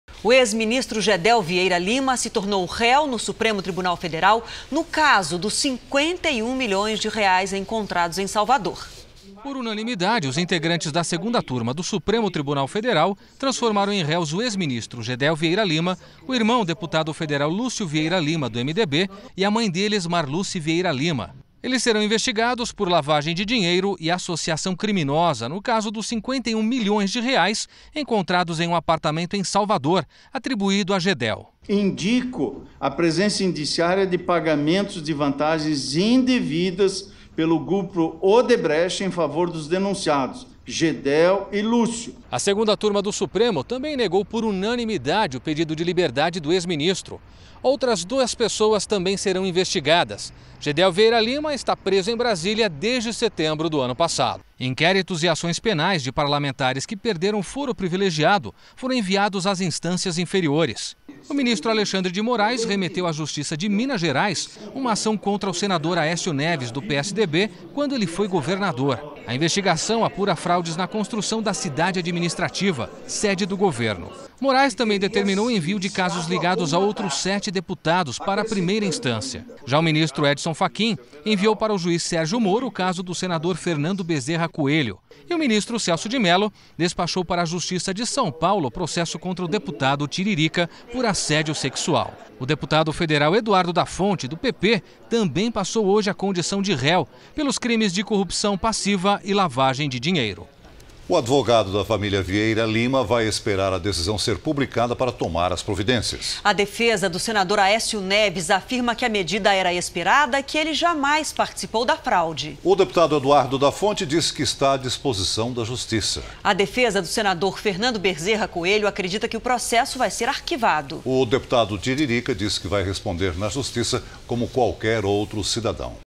Ministro Carlos Marum acusa Judiciário de perseguir Temer
O ministro criticou os vazamentos das informações sigilosas dos inquéritos que investigam o presidente Michel Temer.